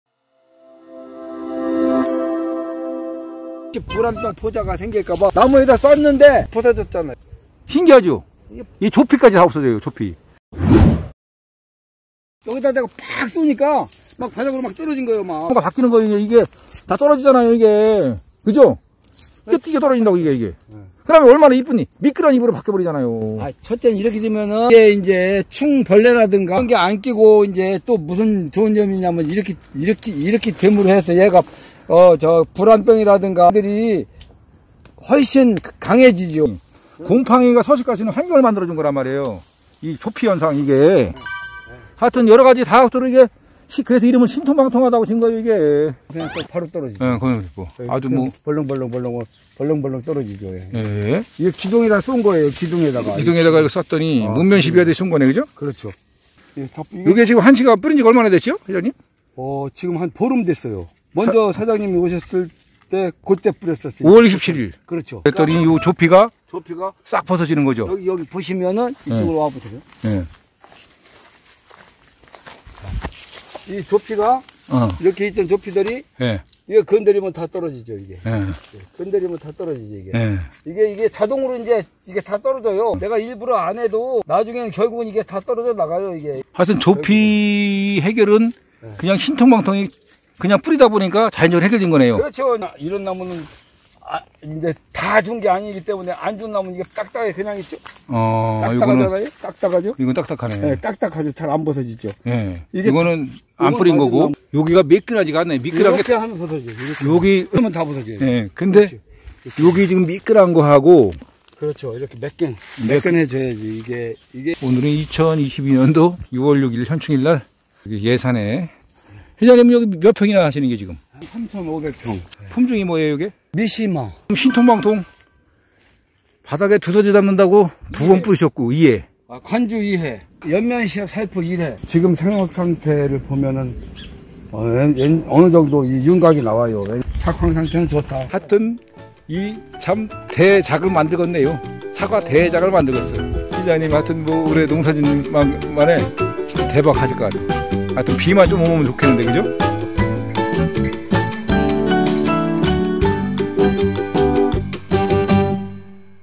과수 조피현상 해결 방법은?
- 네이버 검색어 : 얼라이브 효모농법  (또는) 신통방통 농법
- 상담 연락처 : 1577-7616
(기비 제품은 전화상담으로 구매가능합니다)

[과수 조피현상 해결사 신통방통 농법!]

◇방문장소 : 충남 예산군 고덕면 사과 농가
◇농가 정보 : 3,500평 미시마후지
◇촬영 목적 : 부란병 예방으로 신통방통을 목면시비 하였는데
조피가 너무 잘벗겨져서 조피문제 해결모습 촬영

★조피문제가 심각할 경우 곰팡이, 벌레 등 번식하기 좋은 환경이
조성되어 부란병 등이 발생하게 될 수 있다고 합니다.
1. 우선 수세가 급격히 떨어진다.
2. 생산량이 줄어든다.
3. 과일의 품질이 하락
4. 비대가 정상적으로 되지 않음(사과)
5. 당도가 올라가지 않음(사과)
6. 색깔이 제대로 나지 않음

◇사용 농법 : 신통방통 (STBT 농법)
- 목면시비 : 5월 23일 500배 희석 살포 1회
- 신통방통 관주 2회 실시

◇농가의 말씀

" 건들지 않아도 자연적으로 나무 껍질이 벗겨 떨어진다 "
" 목면시비 하지 않은 사과나무는 껍질이 딱딱한데 반해
목면시비 처리한 나무는 껍질이 우수수 떨어진다 "
"신통방통 목면시비한 나무는 껍질이 떨어진 후 아주 매끈"

"전체적으로 작황이 매우 좋아서 금년 수확이 기대되는 상태"
"잎이 두껍고 색깔이 윤기가 나는 것이 매우 좋다"
"과실 크기가 좋고 사과 모양 윤곽이 잘 형성되고 있다"